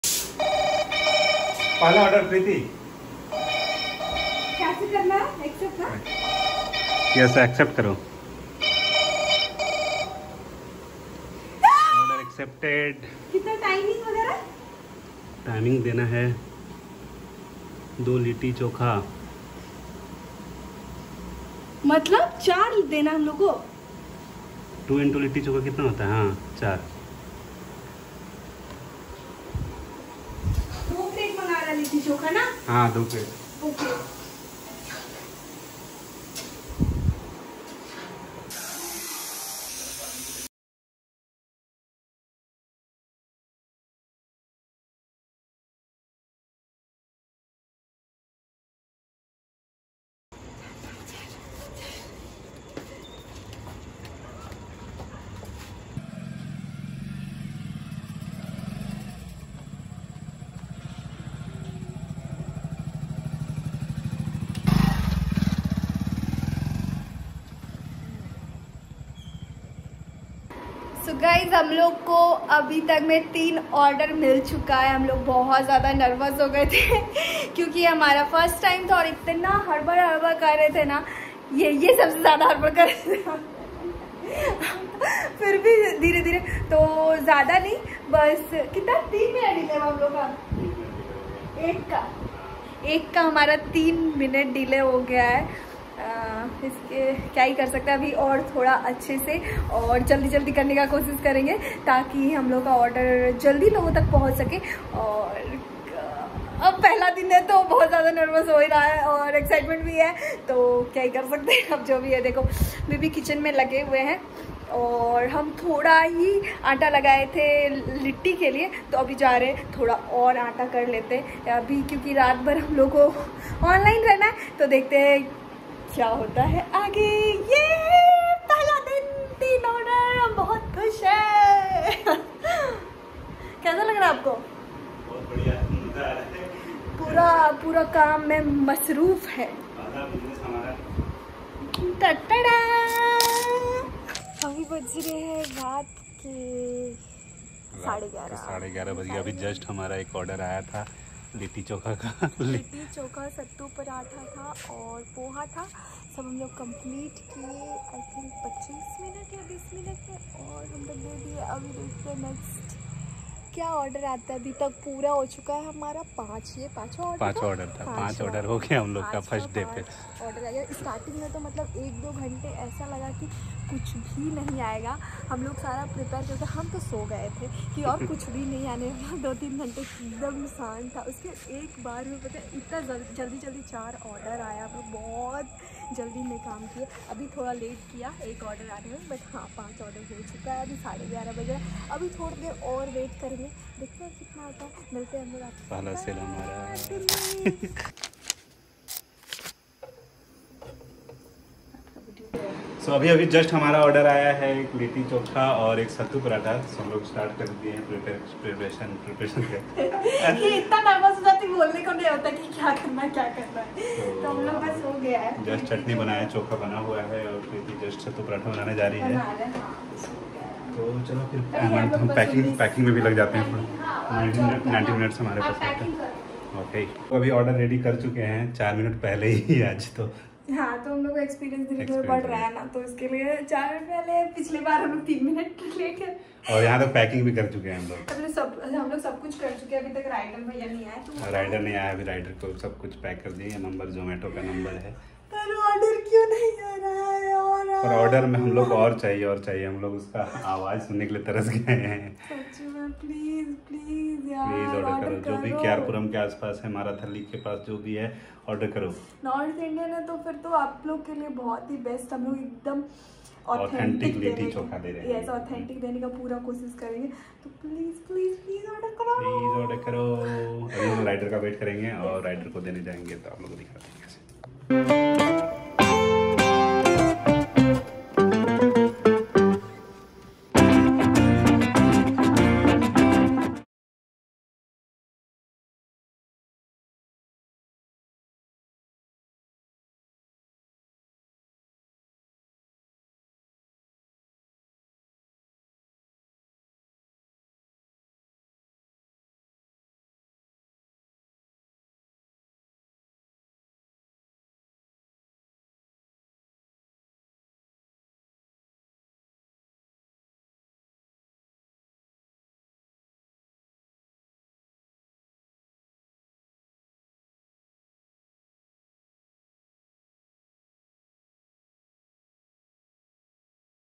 Zomato se aaya pehla order | Cloud kitchen | First order | Episode-2
Hey gys, Mai hun Preeti and maine start kiya hai apna cloud kitchen. and i am going to share here my cloud kitchen journey.

Hamara cloud kitchen ka nam hai Ghar ka Zaika, niche link hai jisse click krke aap direct hamare cloud kitchen ke page pe ja kar order kar sakte ho.